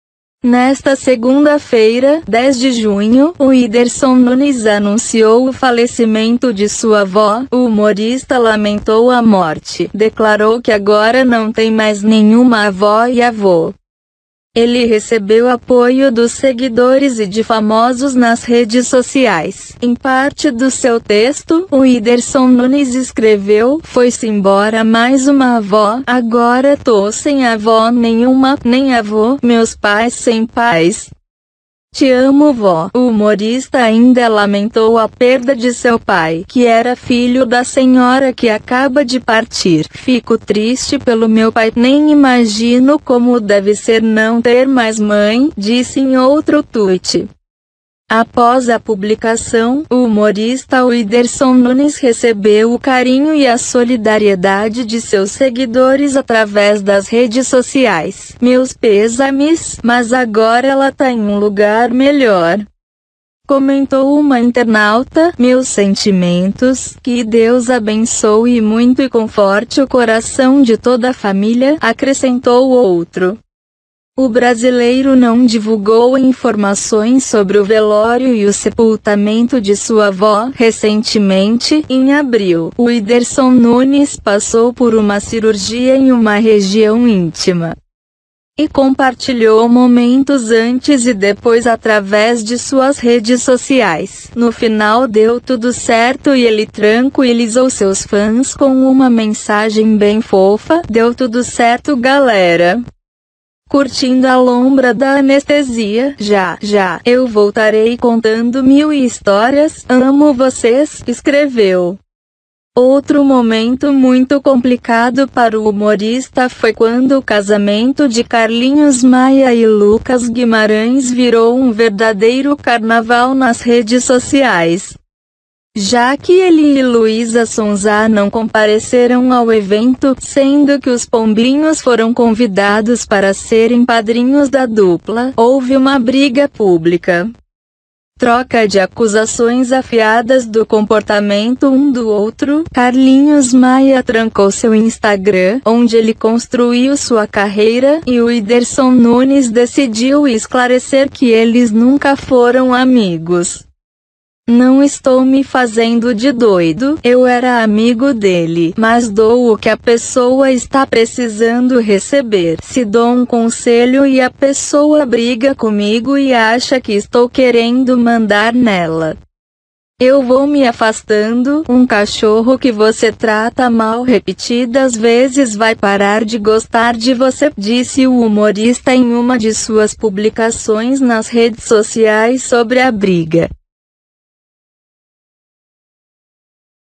Perda do humorista Whindersson Nunes pega todos de surpresa e abala a família
Foi simbora mais uma avó, agora tô sem avó nenhuma, nem avô, meus pais sem pais. Eu visitei ela faz umas duas semanas e prometi que voltaria pra fazer massagem nas pernas dela, a proposta tá de pé pra quando a gente se encontrar. Te amo vó! ❤️

Nesta segunda-feira, 10 de junho, Whindersson Nunes anunciou o falecimento de sua avó. O humorista lamentou a morte, declarou que agora não tem mais nenhuma avó e avô.
Em parte do seu texto, Whindersson Nunes escreveu: “Foi simbora mais uma avó, agora tô sem avó nenhuma, nem avô, meus pais sem pais. Te amo vó!”. O humorista ainda lamentou a perda de seu pai, que era filho da senhora que acaba de partir. “Fico triste pelo meu pai, nem imagino como deve ser não ter mais mãe“, disse em outro tuíte.
Mas agora ela tá em um lugar melhor”, comentou uma internauta. “Meus sentimentos! Que Deus abençoe muito e conforte o coração de toda família!”, acrescentou outro. O brasileiro não divulgou informações sobre o velório e o sepultamento de sua avó.
No final deu tudo certo e ele tranquilizou seus fãs com uma mensagem bem fofa. “Deu tudo certo galera. Curtindo a lombra da anestesia. Já, já, eu voltarei contando mil histórias. Amo vocês”, escreveu.
Outro momento muito complicado para o humorista foi quando o casamento de Carlinhos Maia e Lucas Guimarães virou um verdadeiro carnaval nas redes sociais, já que ele e Luísa Sonza não compareceram ao evento, sendo que os pombinhos foram convidados para serem padrinhos da dupla.
Houve uma briga pública, troca de acusações afiadas do comportamento um do outro. Carlinhos Maia trancou seu Instagram, onde ele construiu sua carreira, e Whindersson Nunes decidiu esclarecer que eles nunca foram amigos. “Não estou me fazendo de doido. Eu era amigo dele, mas dou o que a pessoa está precisando receber. Se dou um conselho e a pessoa briga comigo e acha que estou querendo mandar nela, eu vou me afastando.